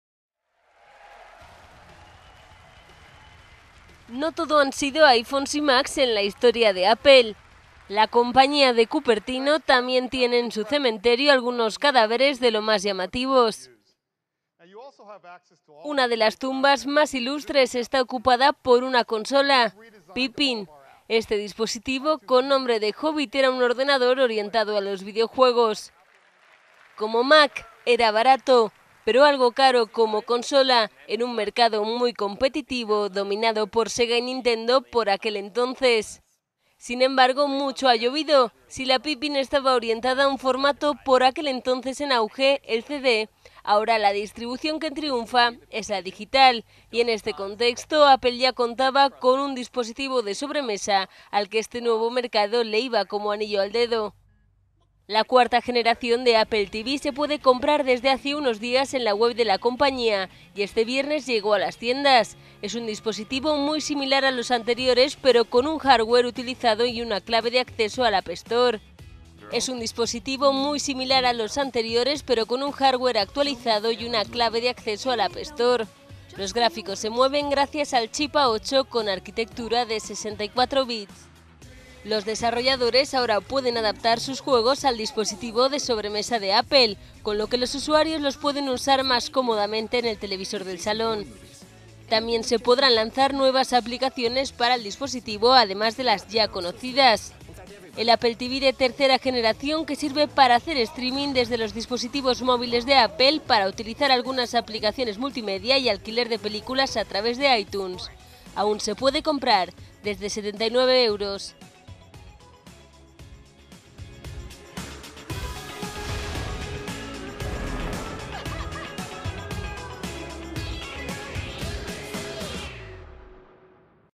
Ya está aquí la segunda "consola" de Apple
No todo han sido iPhones y Macs en la historia de Apple. La compañía de Cupertino también tiene en su cementerio algunos cadáveres de lo más llamativo. Una de las tumbas más ilustres está ocupada por una consola de videojuegos: Pippin.